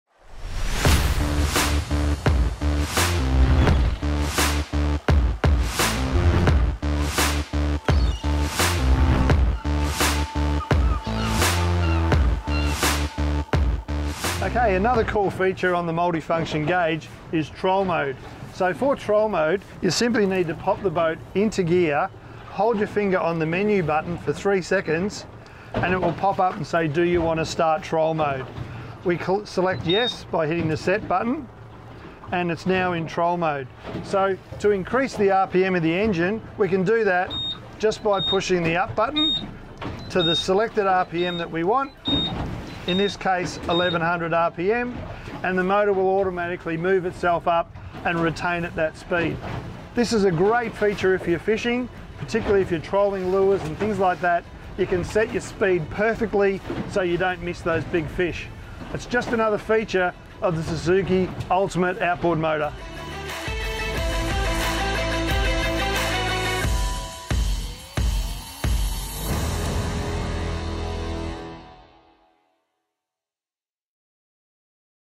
Troll Mode on the Suzuki Multi-Function Gauge (SMG): The Ultimate Outboard Motor
In this video, Suzuki Marine District Aftersales Manager, Rob Ireland, demonstrates how to use Troll Mode with the Suzuki Multi-Function Gauge (SMD).

This system helps the boat run at a certain speed range in low revs, without having to operate the throttle on the boat. It will give you highly precise control at low revs. Available with Suzuki Multi-Function Display (SMD), Suzuki Multi-Function Gauge (SMG), or Troll Mode Switch Panel.
Just another feature of Suzuki, and that’s why we’re The Ultimate Outboard Motor.